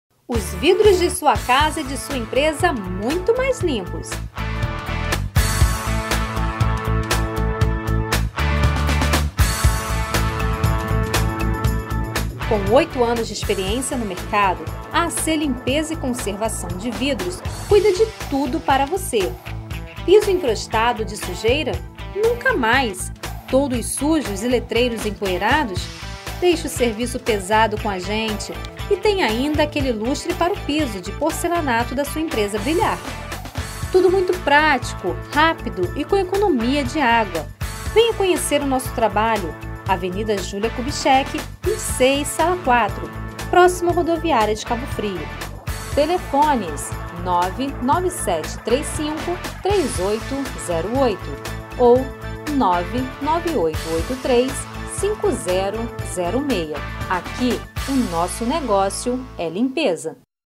Técnica de limpeza de vidros economiza água
AC Limpeza de Vidros e Pisos, em Cabo Frio,  destaca-se no mercado por sua eficiência e comprometimento com os clientes! Um empresa especializada em limpeza de vidros, fachadas, toldos e pisos!